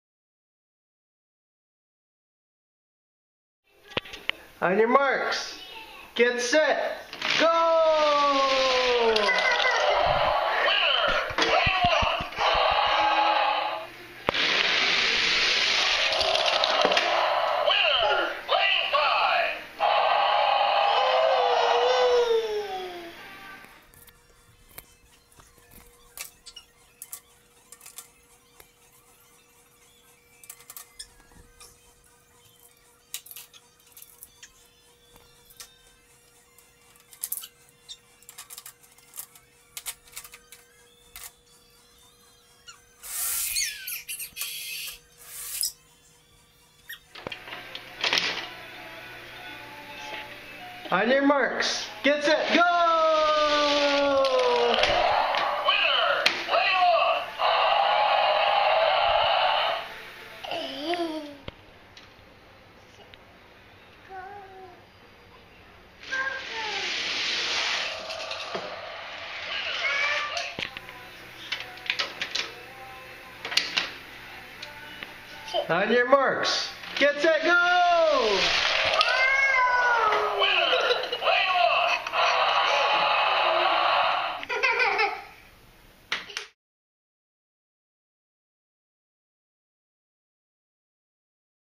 Jonah and his Hot Wheels Super 6-Lane Raceway
Here is Jonah at 22-Months old playing with his Hot Wheels Super 6-Lane Raceway.  He loves this thing and I am surprised how fast he got the hang of setting up the cars.